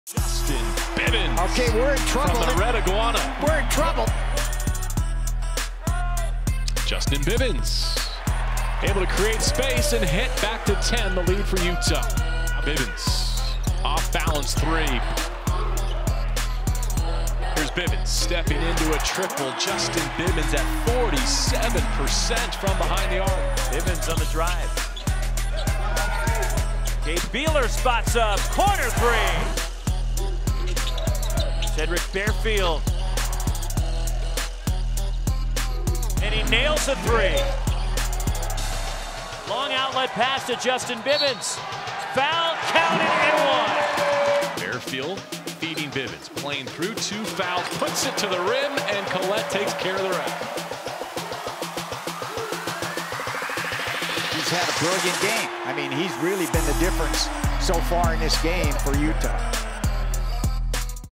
Utah's Justin Bibbins named Pac-12 Men's Basketball Player of the Week
Utah senior guard Justin Bibbins was named Pac-12 Men's Basketball Player of the Week after leading the Runnin' Utes to a weekend sweep of the Washington schools.